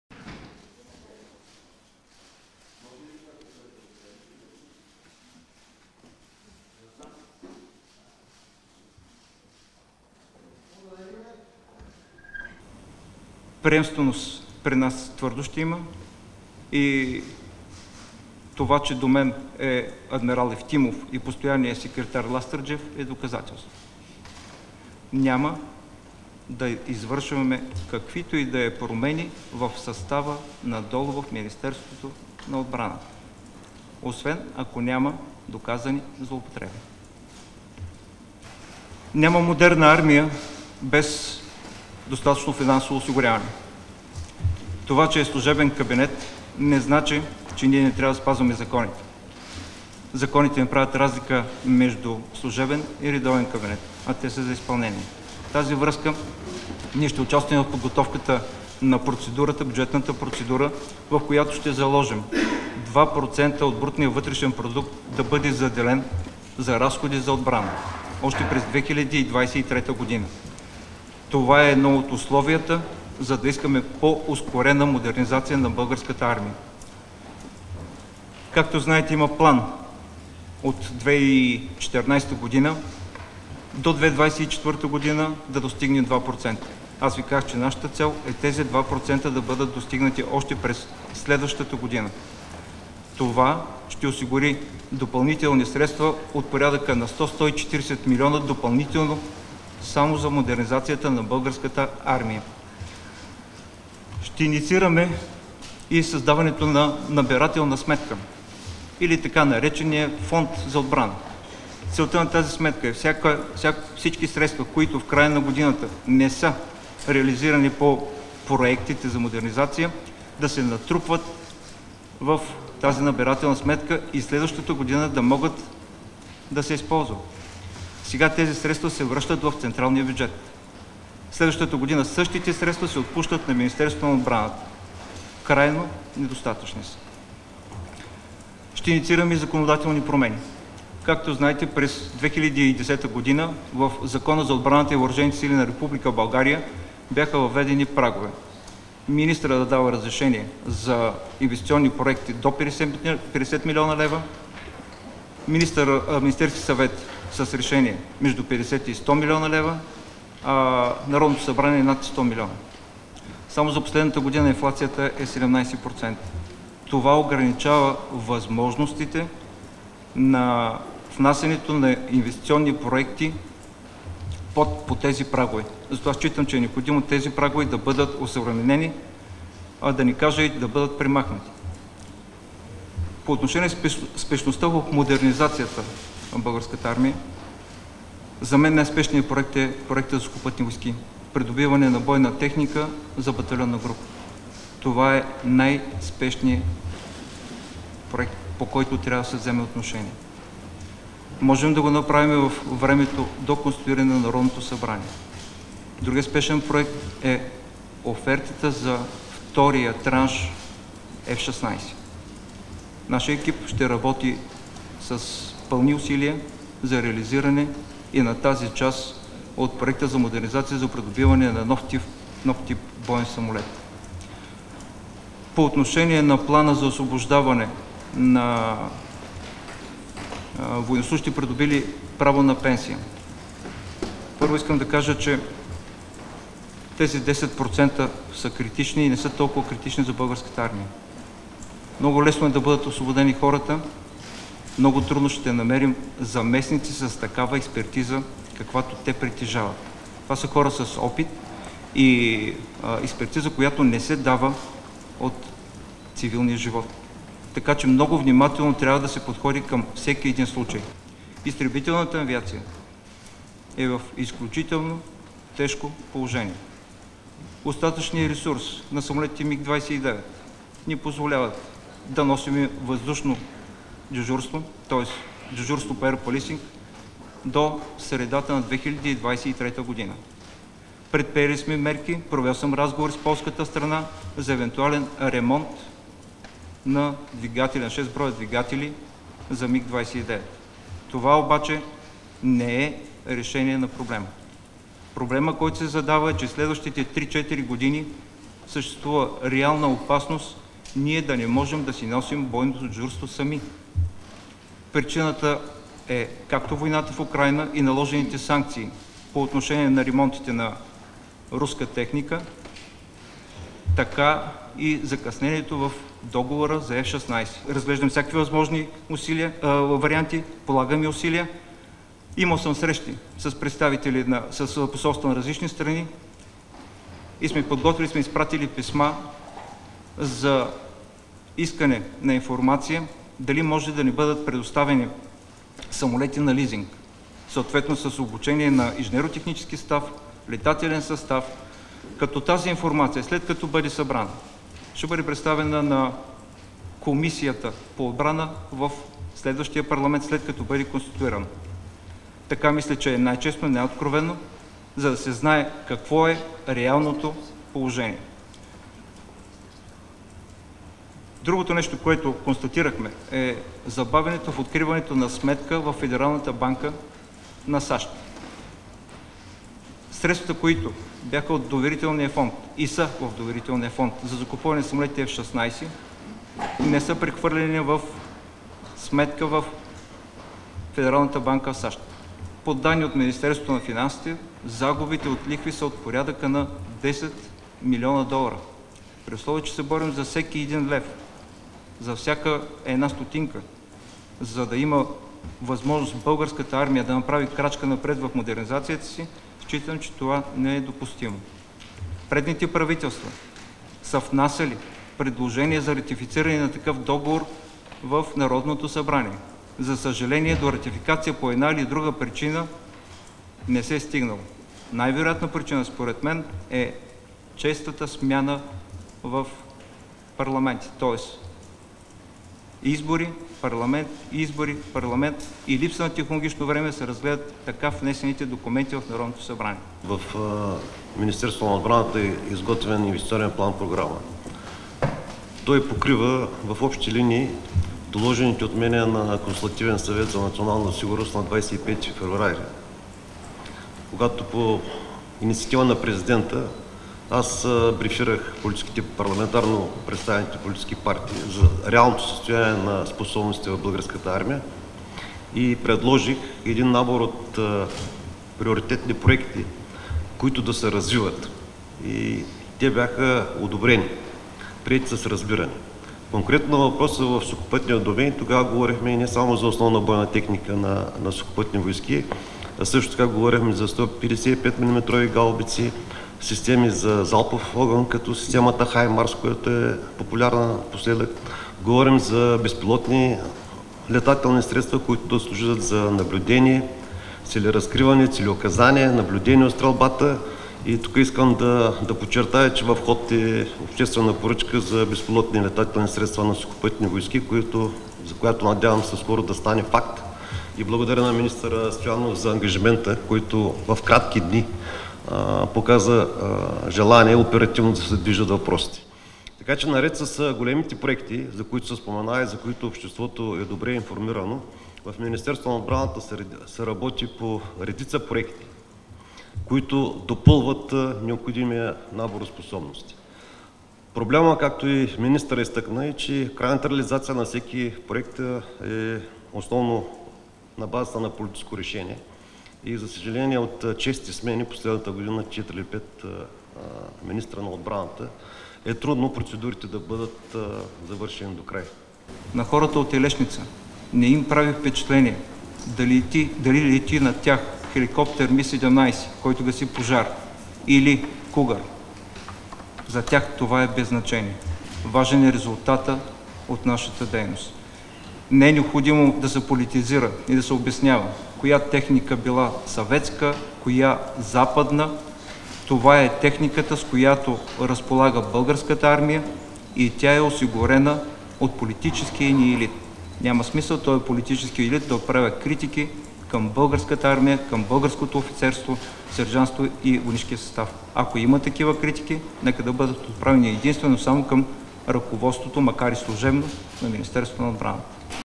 България загубила $10 млн. от забавена ратификация на договор със САЩ
Според служебния министър на отбраната Димитър Стоянов това е недопустимо, ако страната ни се "бори за всеки лев" за модернизацията на армията.